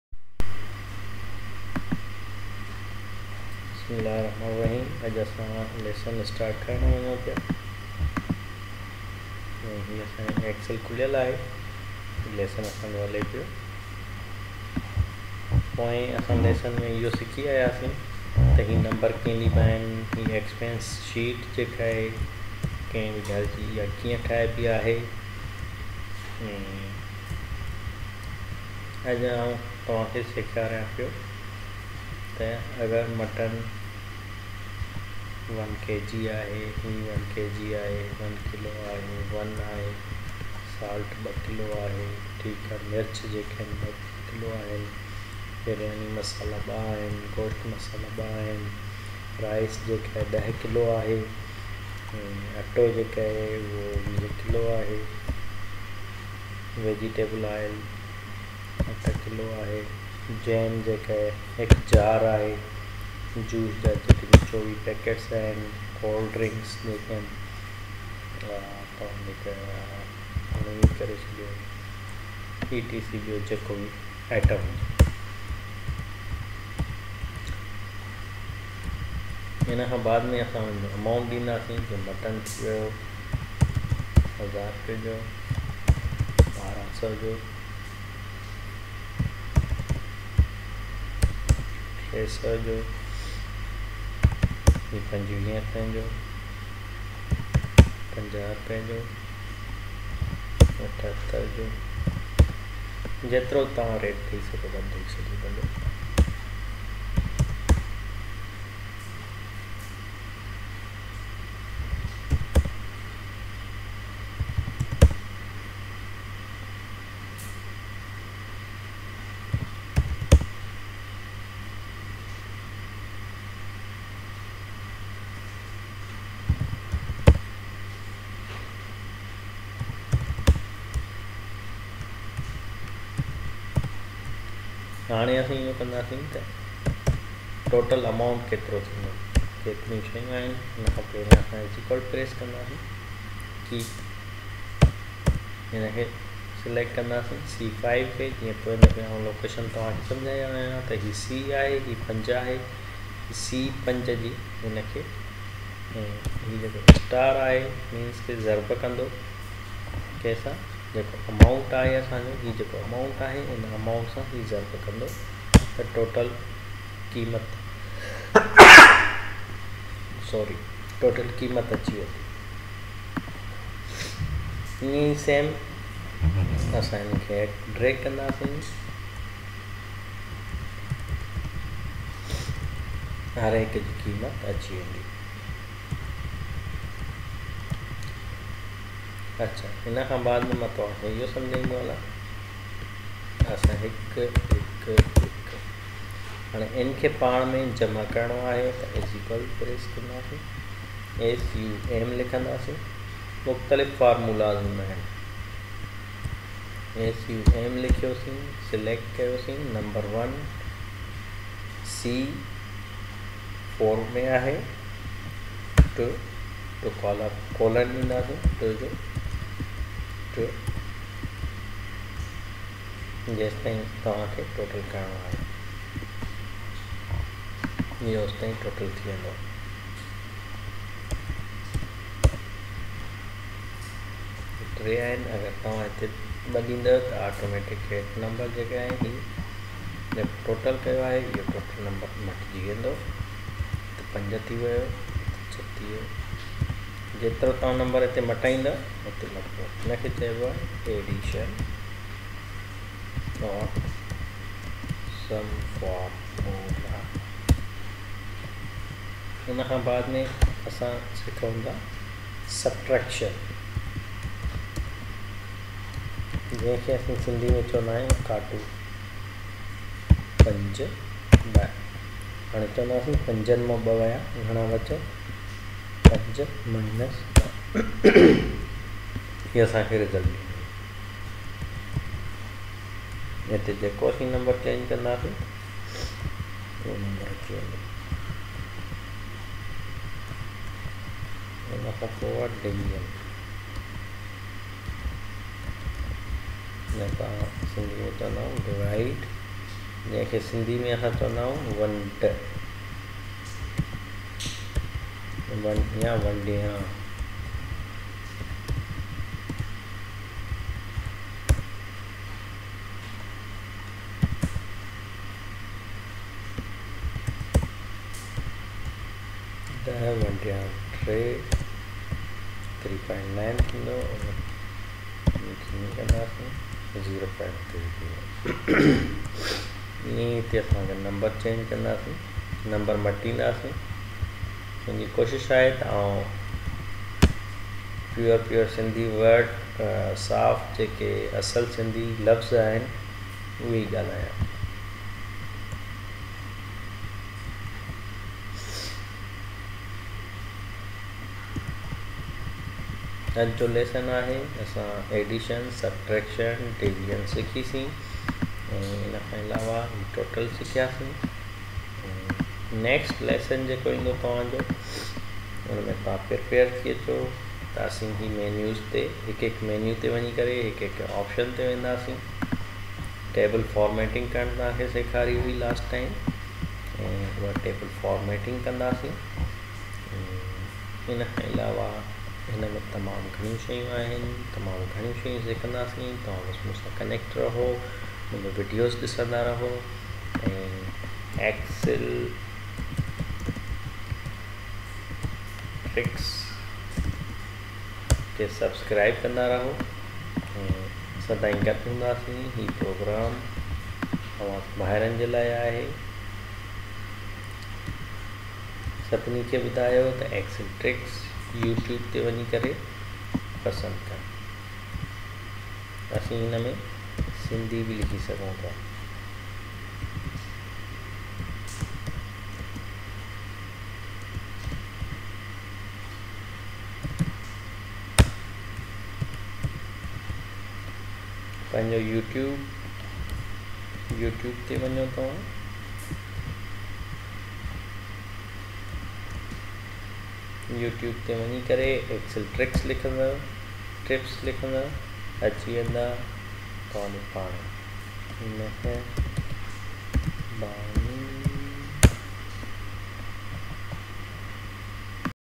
MS Excel SUM Formula Lesson Two
Microsoft Excel - SUM Formula - Lesson Two learn in Sindhi Language. In this video deliver to you Addition, Subtraction and Multiplication.